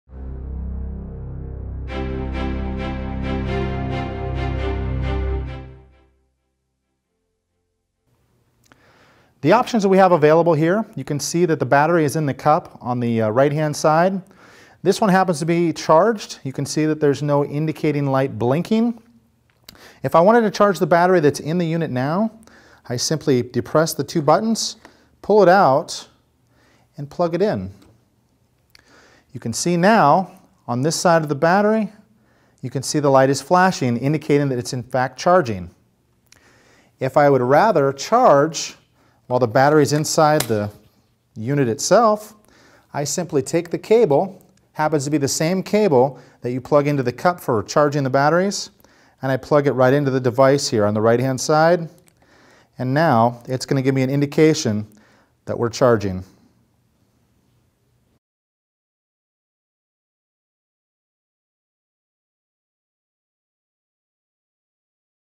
Battery Charging Options for Your Fluke Ti400, Ti300 and Ti200 Infrared Camera
A quick overview of your battery charging options for your Fluke Ti400, Ti300 and Ti200 infrared camera.

You can learn more about Fluke infrared cameras on our